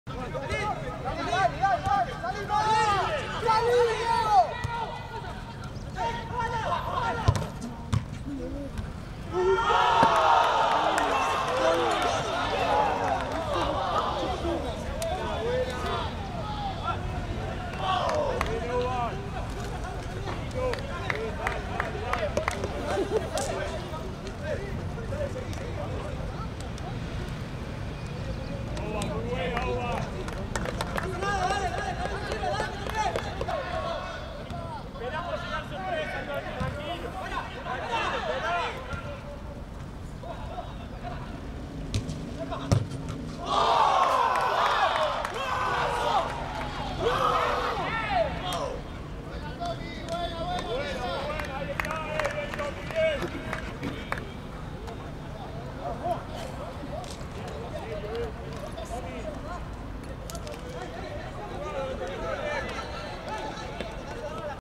Reserva ⚽️ El doblete de Londoño Bedoya vs. Central Córdoba 💥
El goleador de la Reserva volvió a marcar por duplicado y River sumó tres puntos más en Santiago del Estero: fue triunfo 2-1 para el puntero invicto del torneo frente a Central Córdoba. 👏